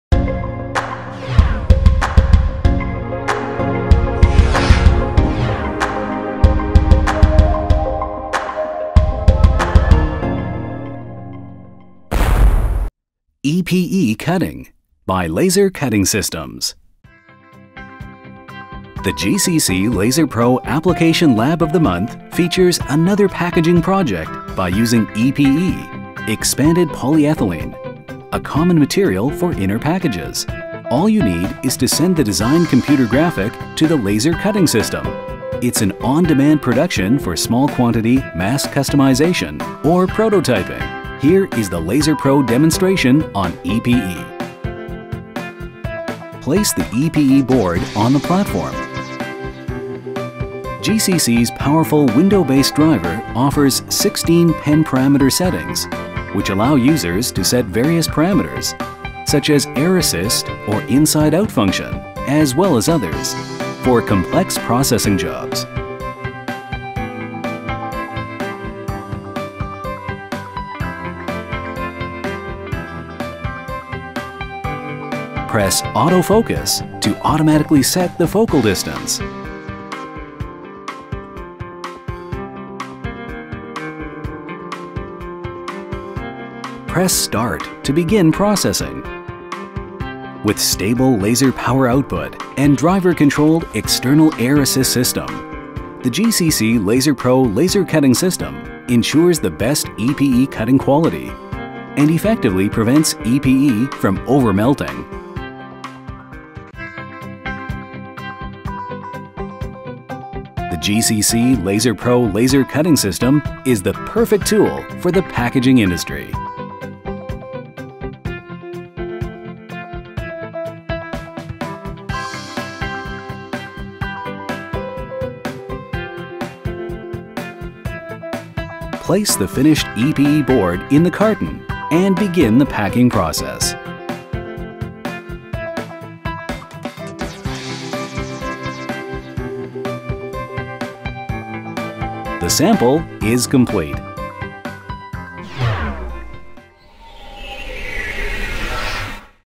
GCC LaserPro---EPE Cutting by Laser Cutting Systems
The GCC LaserPro Application Lab has previously introduced various packaging projects. Laser is an efficient and practical tool for processing both outer and inner packages. This month, the GCC LaserPro Application Lab characterizes another packaging project by using EPE (Expanded Polyethylene), a material with exceptional elasticity and impact strength that is widely used in protective packaging.

Traditionally, EPE is cut through casting molds tailored to the specific shape and size requests. It is a huge bill to face for small number production. Laser cutting systems eliminate the worries as all you need is send designed images to the system to allow mass production, saving the cost of casting molds. Here is the LaserPro demonstration on EPE: